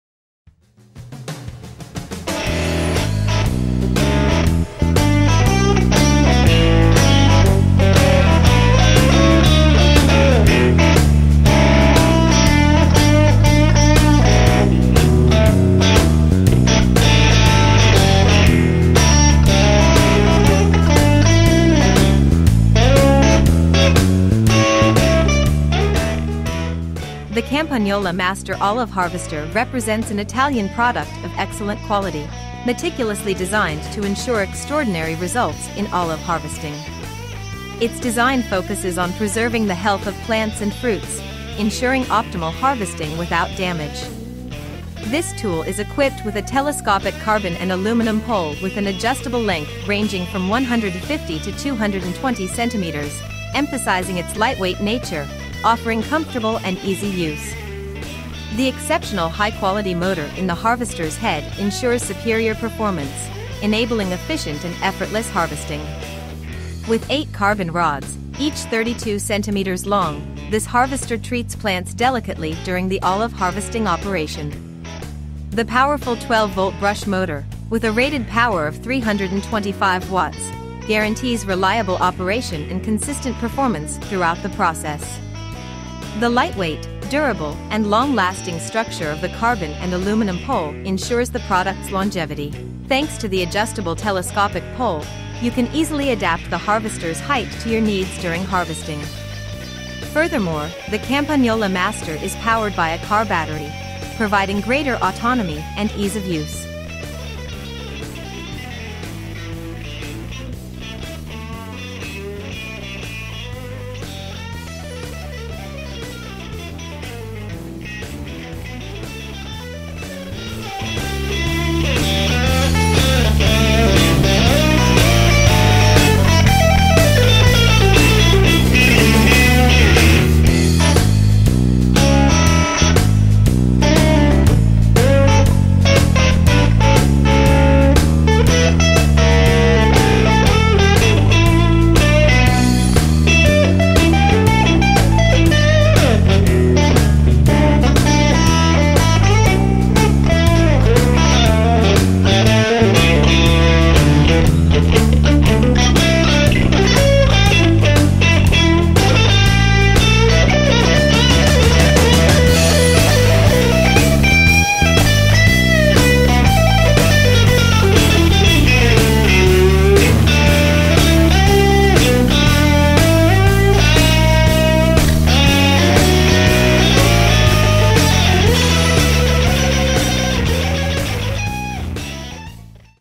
Presentation and assembly of the Campagnola MASTER - Battery Electric Harvester 150-220 cm
Here is a presentation and assembly video of the Campagnola MASTER - Battery Electric Harvester - Carbon and aluminium pole 150-220 cm made directly by us at AgriEuro to show you all the main features.
0:00-0:05 Intro
0:05-0:11 360° of the product
0:11-0:25 Unboxing
0:32-1:55 The machine
1:55-2:00 Spare parts order
2:00-2:51 Assembly guide
2:51-2:55 360° of the product
2:55-3:02 Outro

The Campagnola MASTER olive harvester is a quality Italian product, carefully designed to guarantee exceptional results in olive harvesting. Its design is focused on preserving the health of plants and fruits, ensuring an optimal harvest without damage.

Equipped with a 150/220 cm telescopic carbon and aluminium rod, the MASTER harvester is characterised by its light weight, offering comfortable and easy operation. The high quality motor in the head ensures high performance, allowing efficient and effortless harvesting.

With 8 carbon rods, each 32 cm long, this harvester treats the plants gently during olive harvesting.

The powerful 12 V brush motor offers a nominal power of 325 W, ensuring reliable operation and consistent performance during harvesting. The lightweight, strong and durable structure of the carbon and aluminium pole guarantees a long life.

With its telescopic pole, you can easily adjust the height of the harvester to your needs when harvesting. The Campagnola MASTER is equipped with a car-type battery motor, which provided increased autonomy and ease of use.

The Campagnola MASTER harvester is the ideal choice for olive harvesting, offering superior performance and reliability.